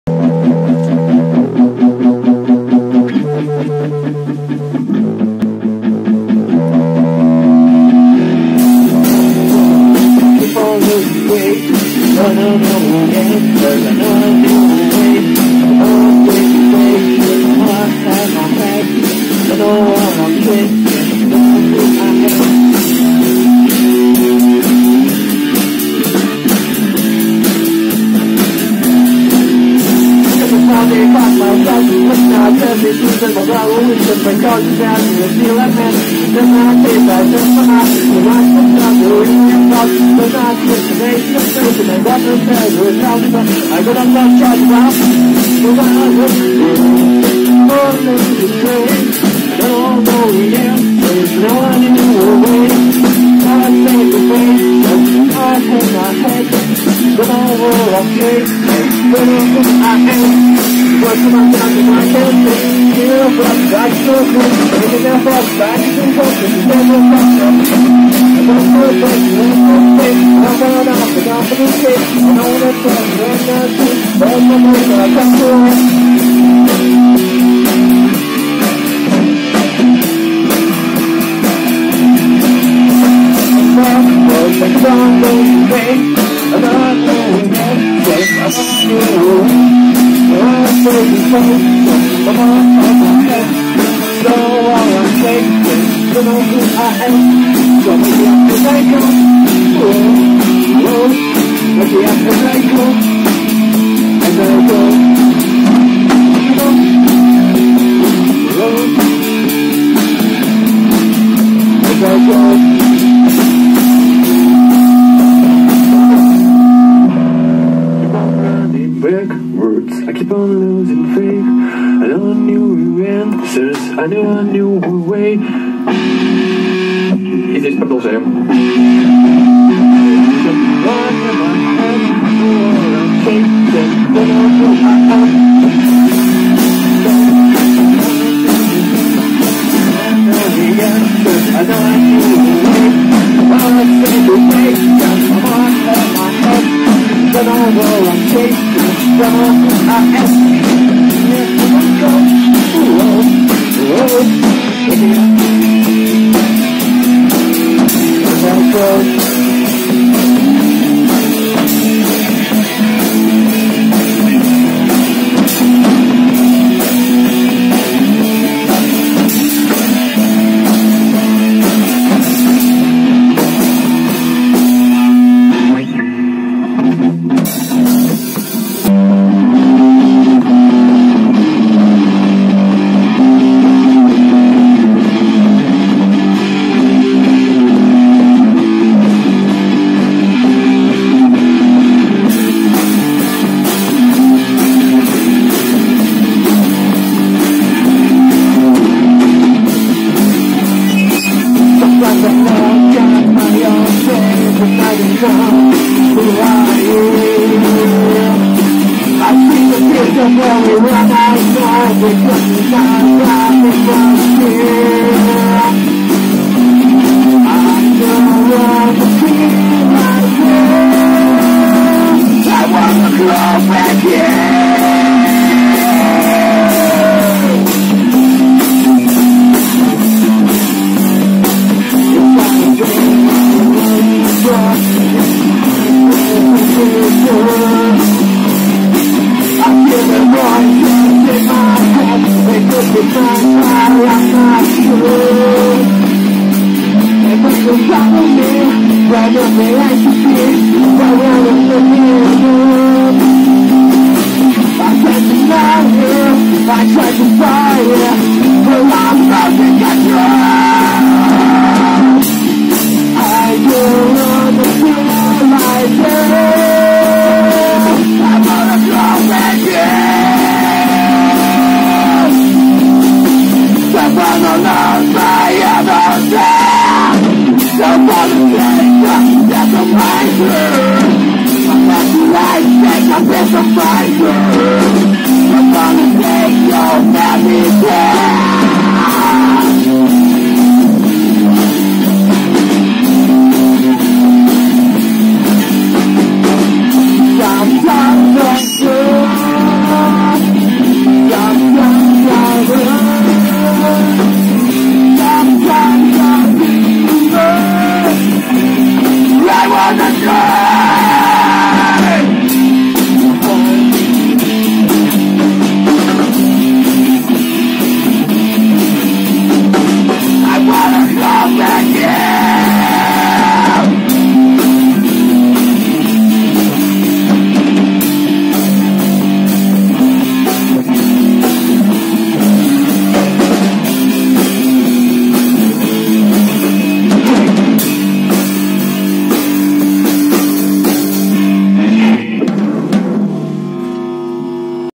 The 3th "IMMORTALITY" rehearsal
2020/01/29

It was an unimaginably deranged rehearsal! it was very energetic and driving! but I won't show you all the songs in this video. 😉 if this video gets 50 likes, then you are good!